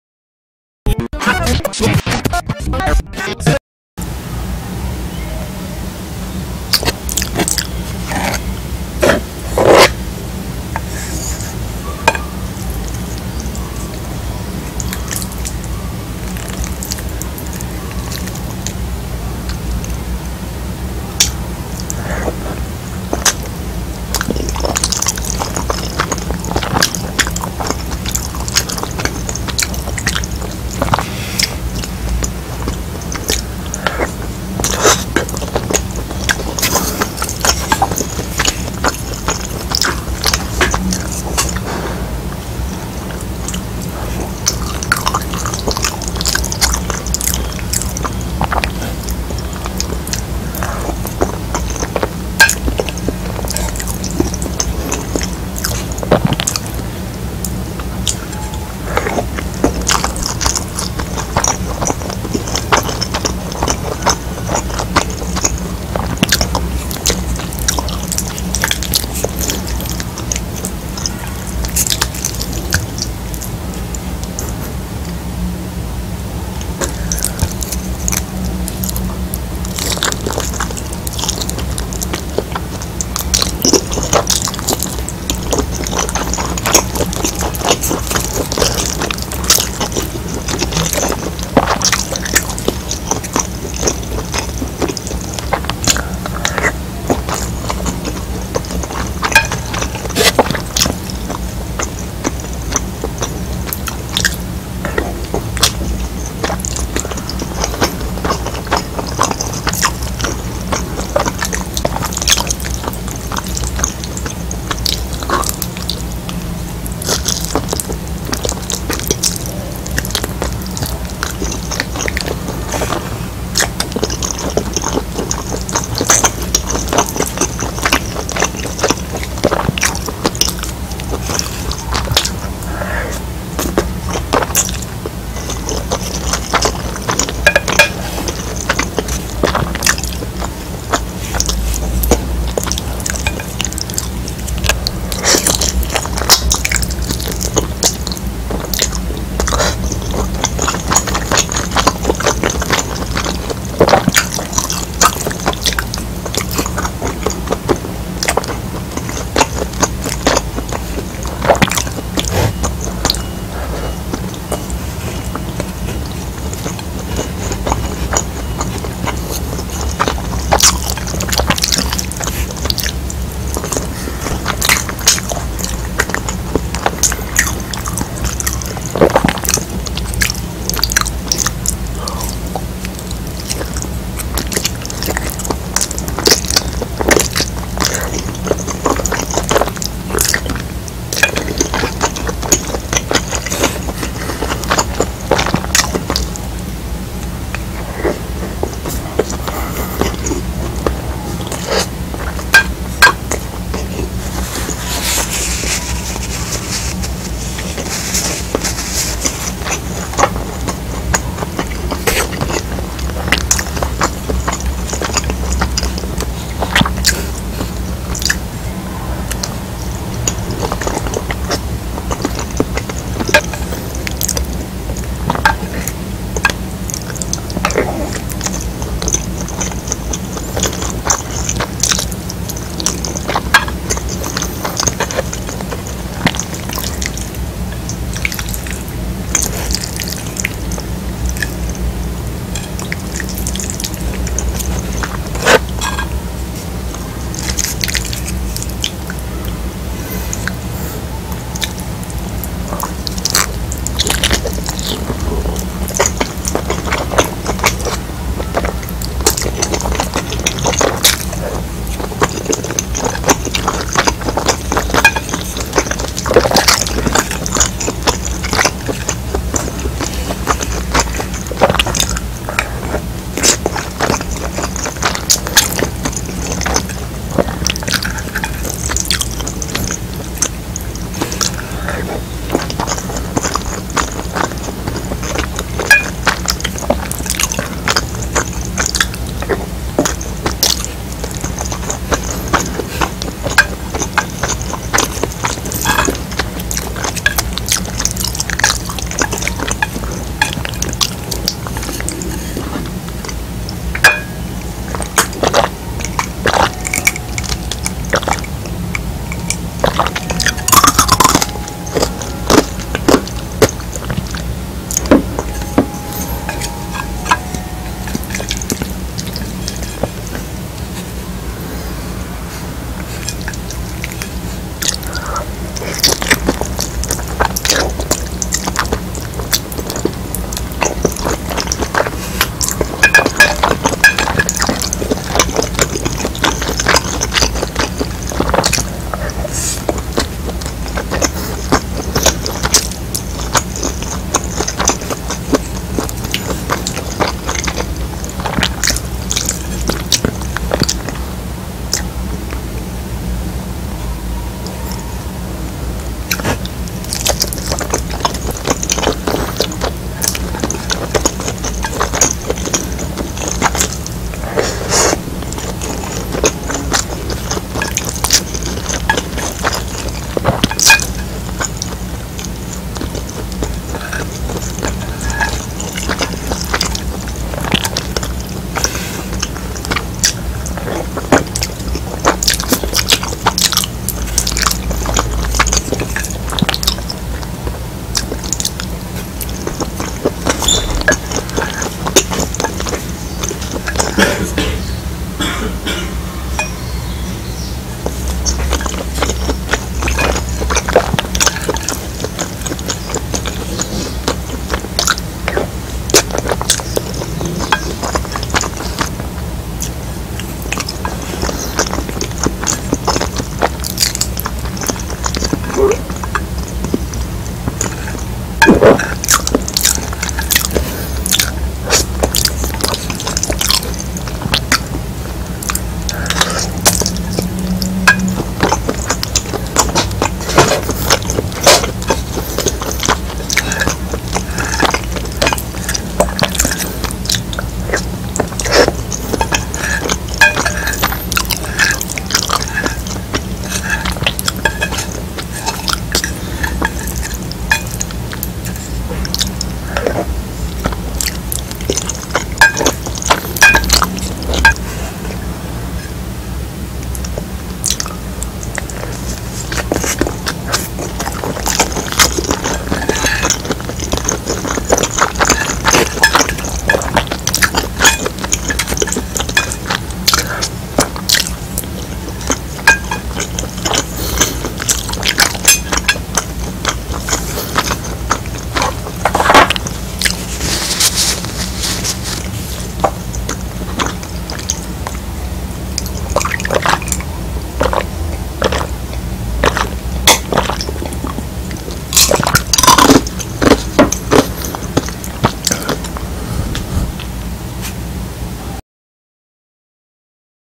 ASMR Eating Sounds | Chicken Steak With Curry Sauce Rice | MAR ASMR
Hello Guys !! Welcome To MAR ASMR !! I Will Eat Chicken Steak With Curry Sauce Rice This Time !! Hope You Soft Enjoy This Sticky And Chewy Eating Sounds. If You Do Not Like The Eating Sounds, Plz Do Not Watch This Video. THANK YOU !!!